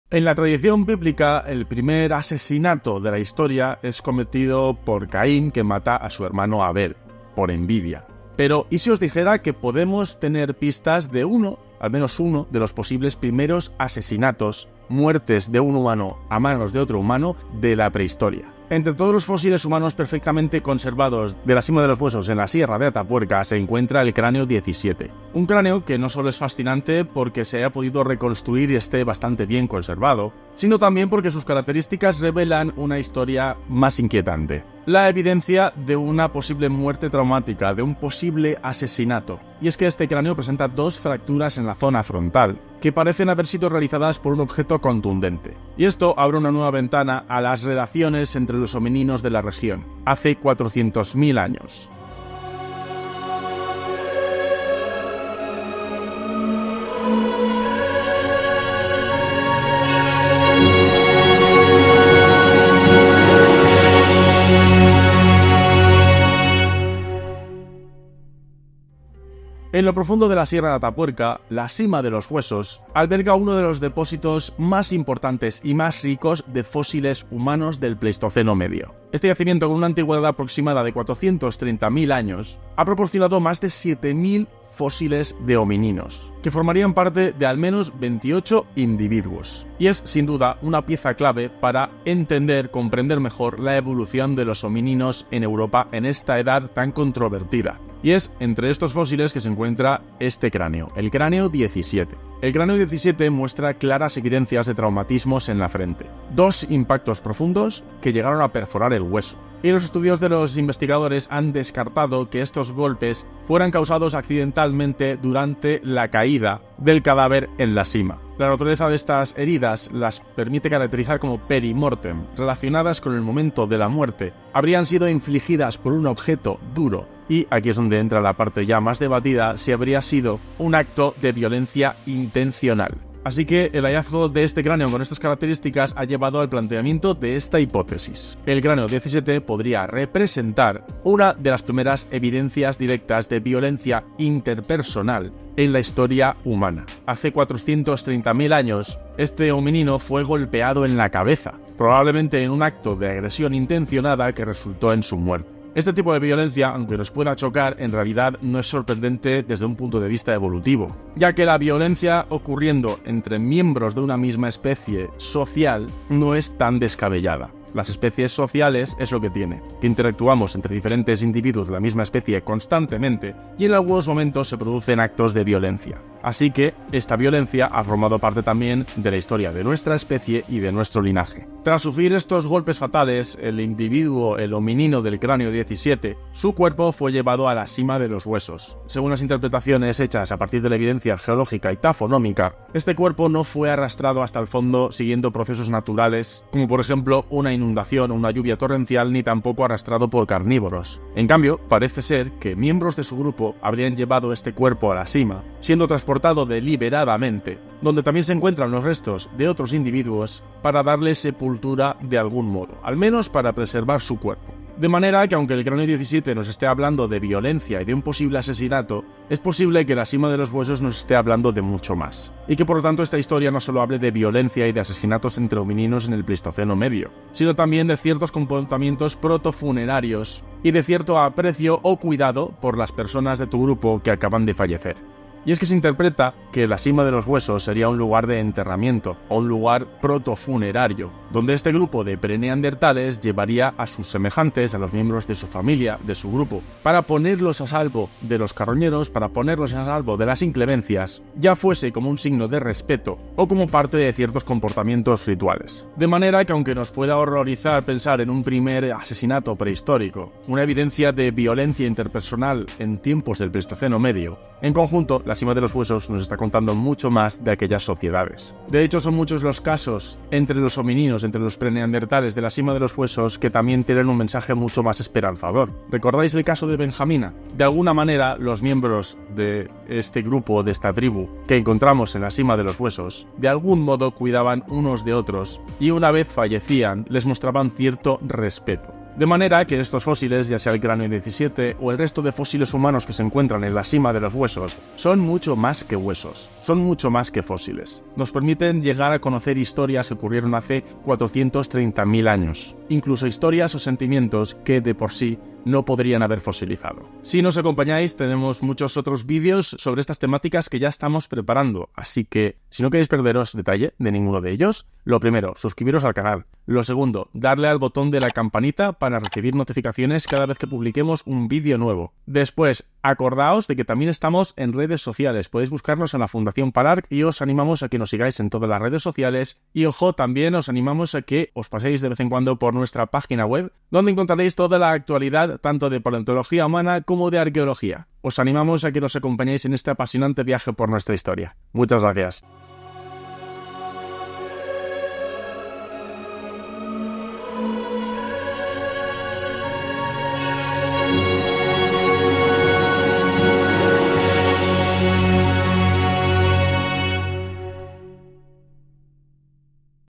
¿Un Asesinato de Hace 430.000 Años? El Caso del Cráneo 17 en Atapuerca
¿Qué secretos esconde el Cráneo 17 encontrado en la Sima de los Huesos en Atapuerca? Este fósil, con más de 430.000 años de antigüedad, nos revela un caso fascinante de violencia prehistórica: dos fracturas en su frente, causadas por un objeto contundente, sugieren un asesinato intencional en el Pleistoceno Medio. Sin embargo, este hallazgo no solo muestra violencia interpersonal, sino también un posible comportamiento funerario, ya que los restos parecen haber sido depositados deliberadamente.

Acompáñanos en este viaje al pasado, explorando la complejidad social y simbólica de nuestros ancestros. Analizaremos cómo el Cráneo 17 nos ayuda a entender las primeras formas de violencia y rituales en la historia humana.